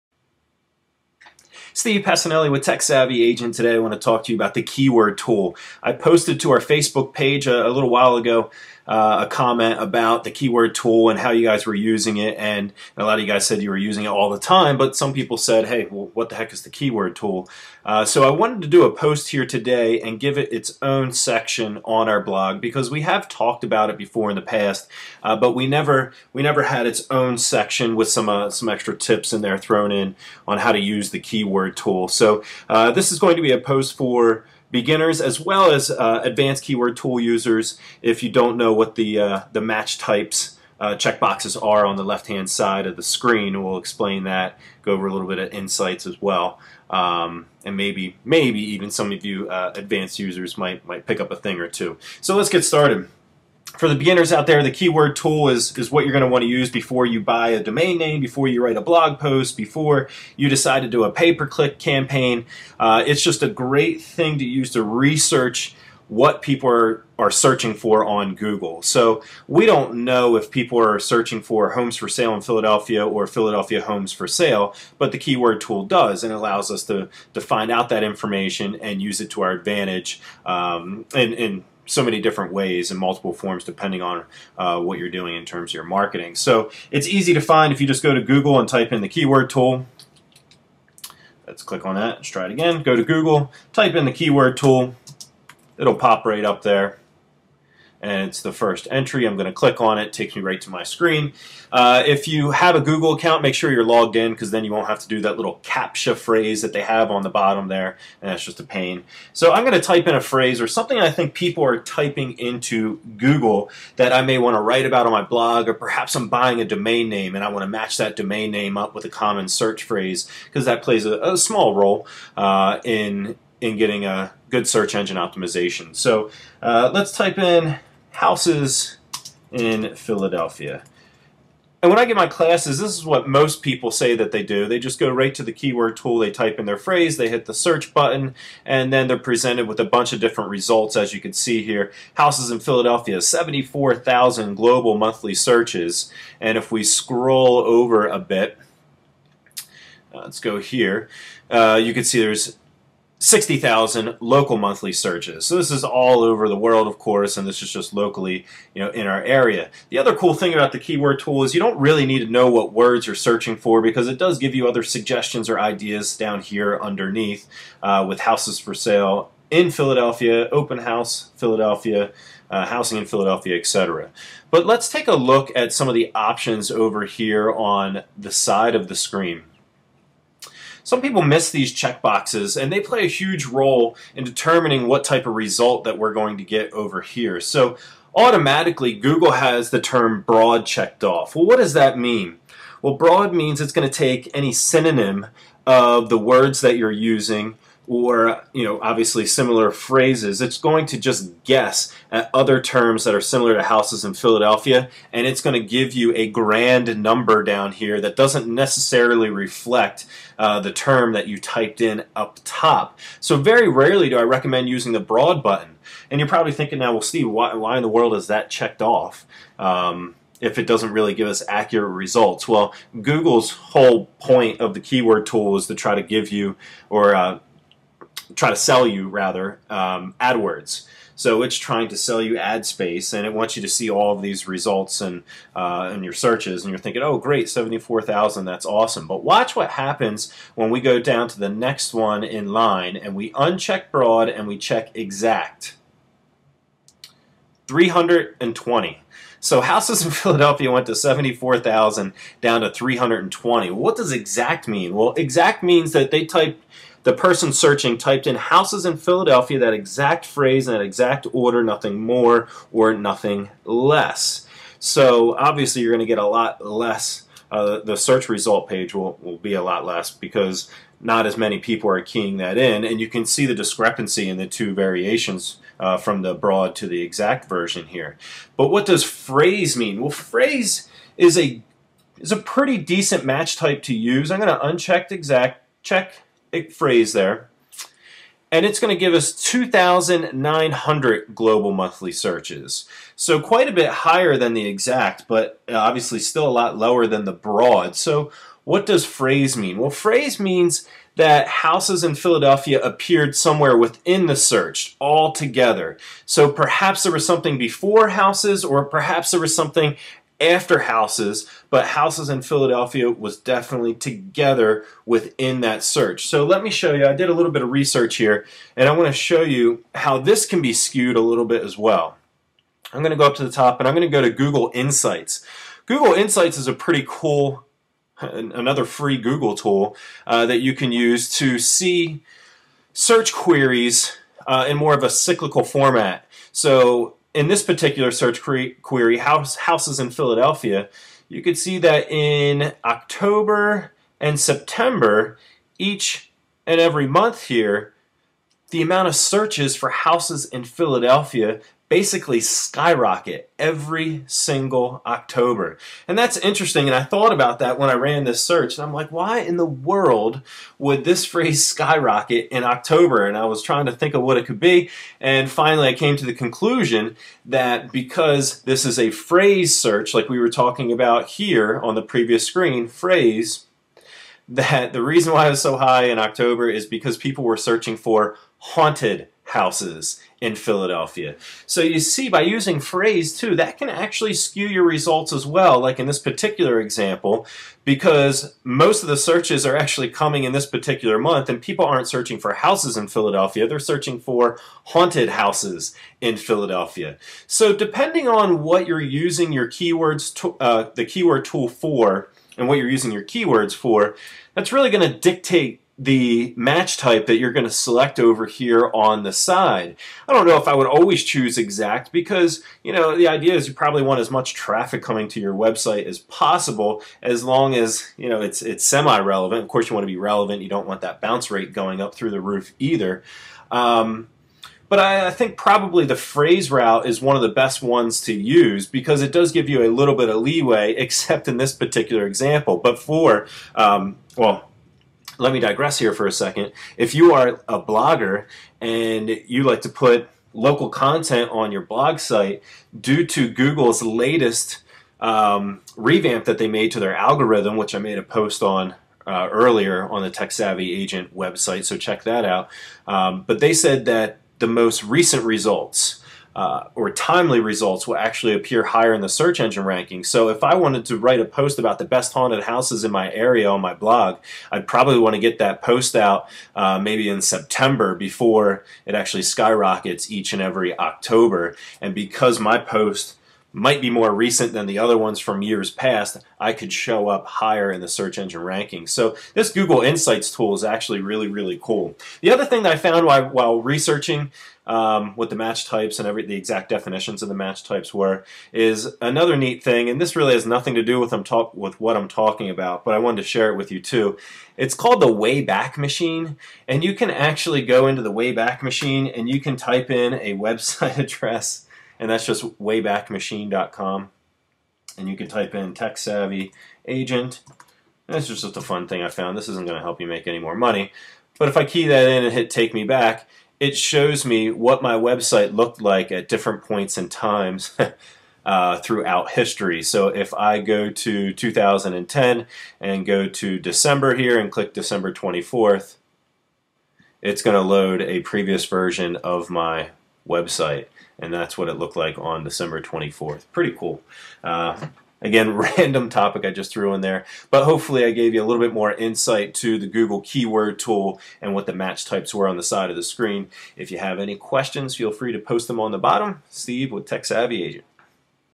Keyword Tool Match Types
Keyword Tool Match Types and how they affect your results